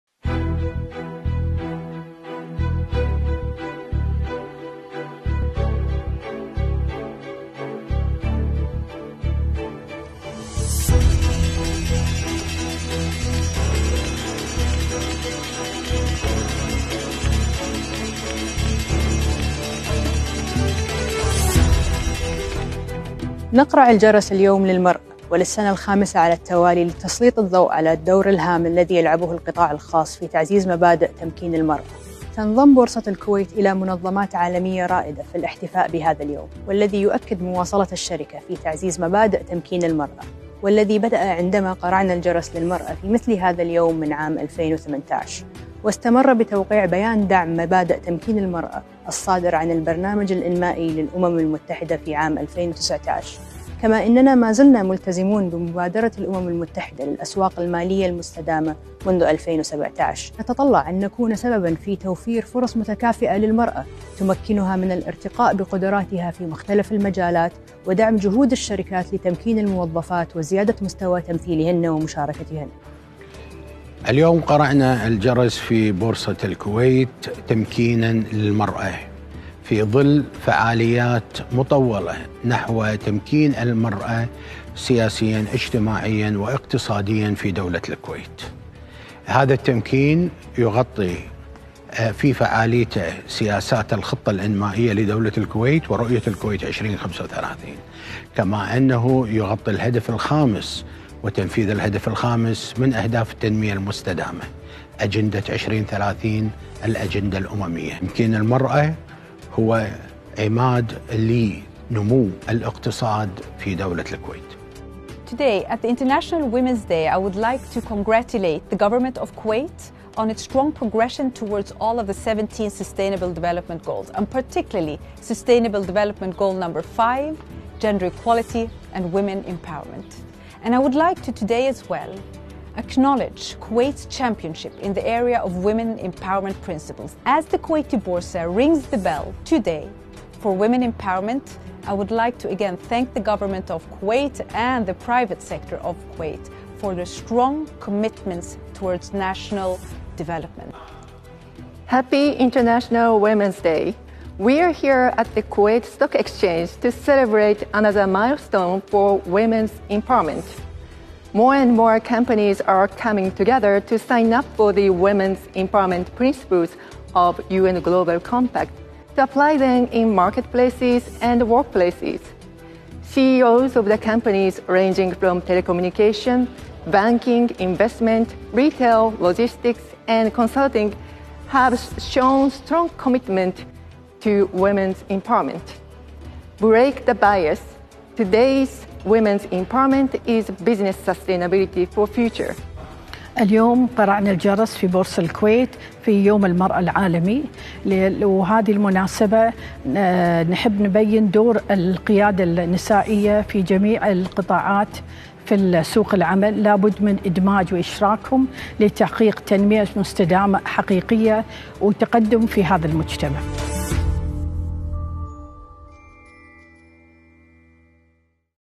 Boursa Kuwait, GSSCPD and the United Nations rings the bell on International Women’s Day
‏الكويت تقرع الجرس لتعزيز تمكين المرأة في يومها العالمي في فعالية نظّمتها بورصة الكويت وهيئة الأمم المتحدة بالتعاون مع الأمانة العامة للمجلس الأعلى للتخطيط لزيادة الوعي في مجالات تمكين المرأة في القطاع الخاص ورفع الوعي حول مبادئ تمكين المرأة.